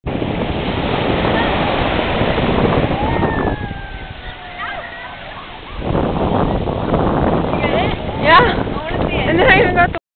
supernatural written on the beach
we wrote supernatural in the sand just for you monty :) then the waves washed it away